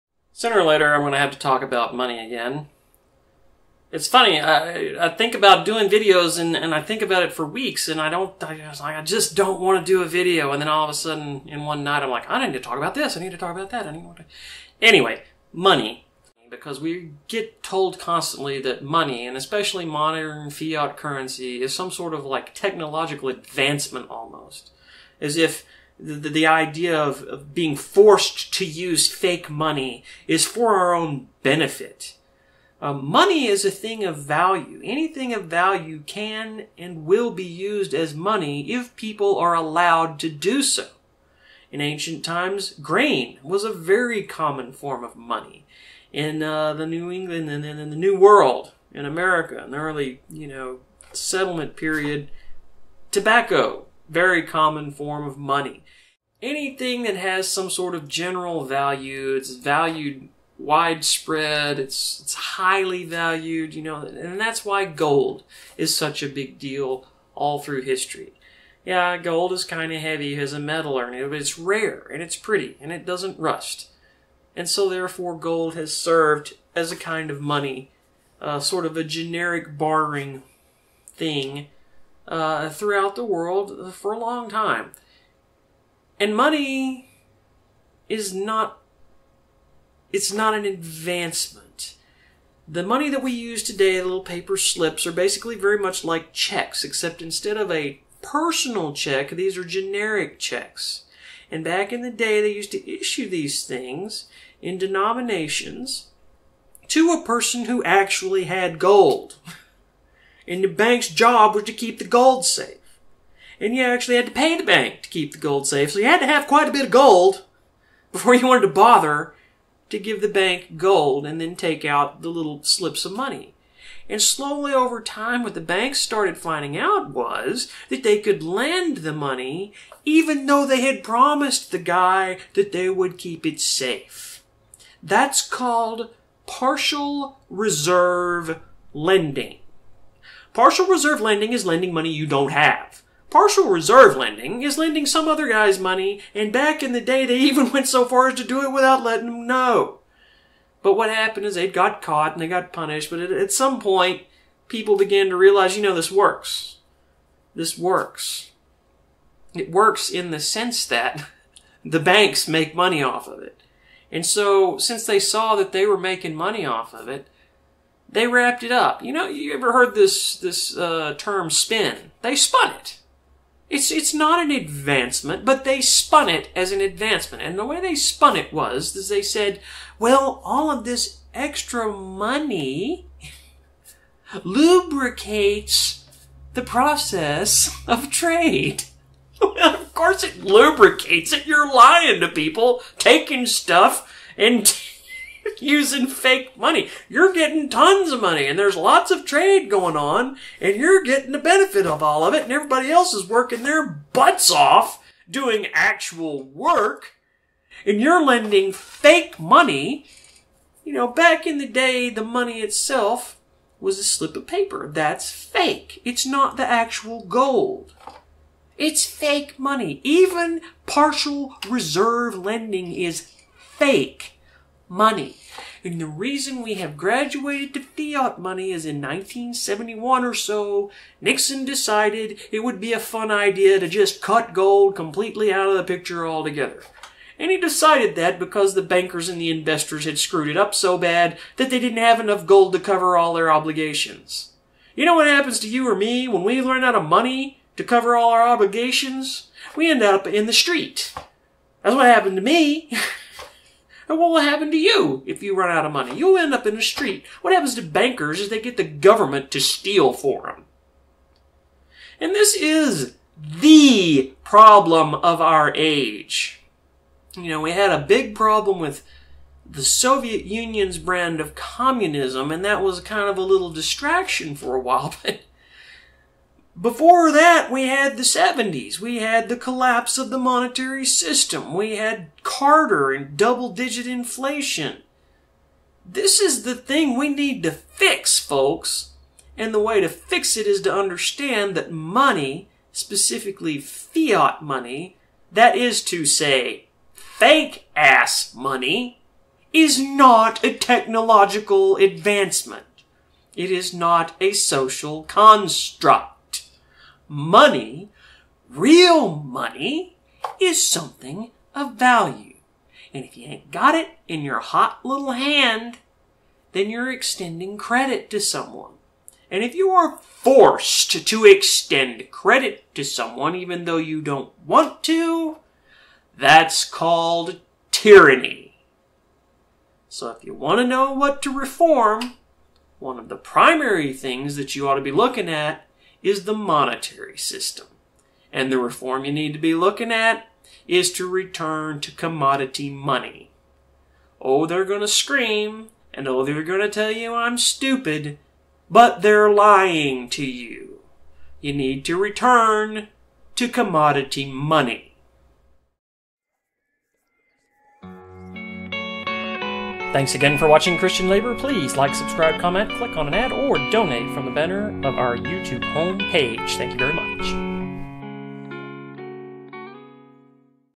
Return to Commodity Money (not monetizable why?)
Revisiting the Three Pillars of Fascism. The propaganda is everywhere. I even saw it in a video game recently. Fiat currency is an economic ideological advancement.

No it is not. Fiat currency is a lie.